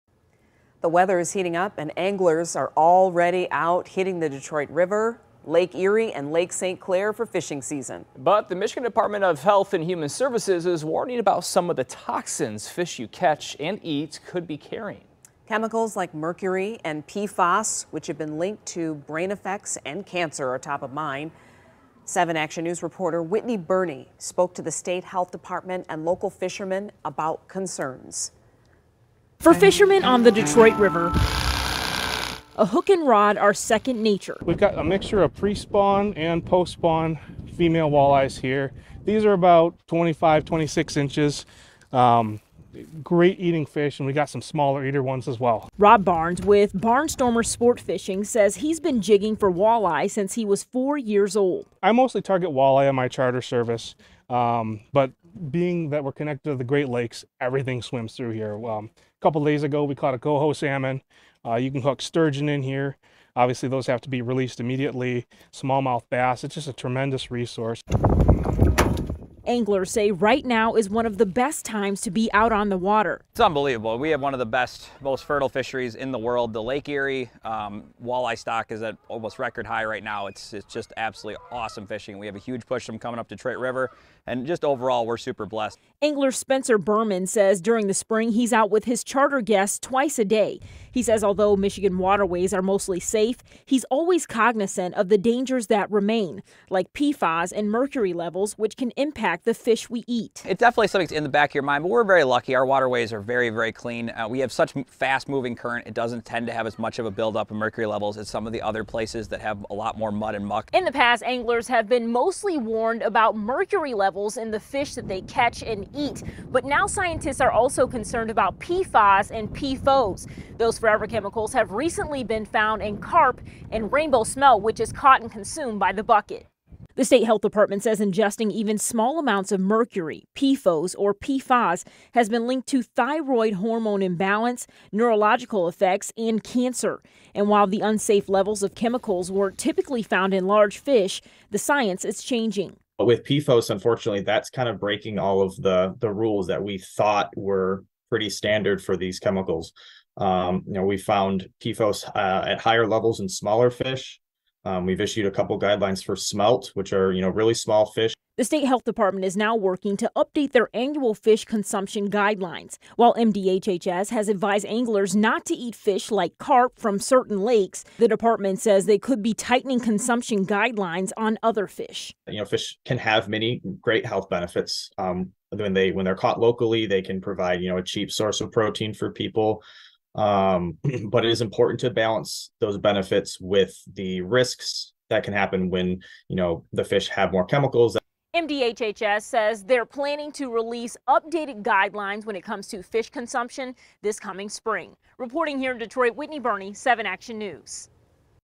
Fish consumption guidelines updated amid PFAS concerns
The state health department says ingesting even small amounts of mercury, PFOS or PFAS has been linked to thyroid hormone imbalance, neurological effects, cancer among other illness and disease.